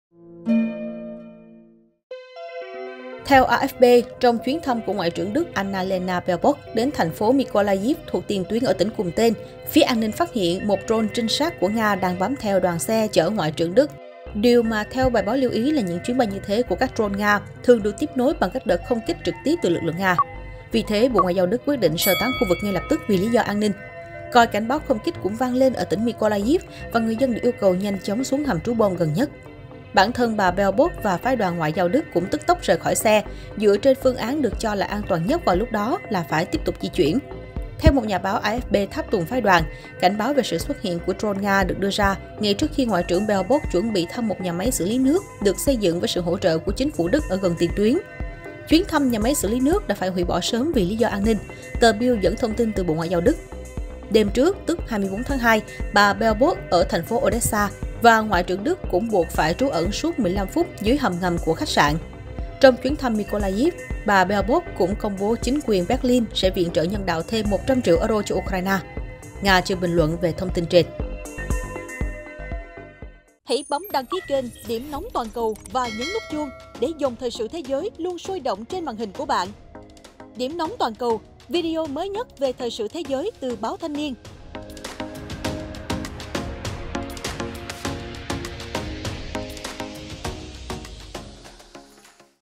UAV Nga bám theo đoàn xe Ngoại trưởng Đức tại Ukraine?
Theo AFP, trong chuyến thăm của Ngoại trưởng Đức Annalena Baerbock đến thành phố Mykolaiv thuộc tiền tuyến ở tỉnh cùng tên, phía an ninh phát hiện một UAV trinh sát của Nga đang bám theo đoàn xe chở ngoại trưởng.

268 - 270 Nguyễn Đình Chiểu, Phường Võ Thị Sáu, quận 3, TP HCM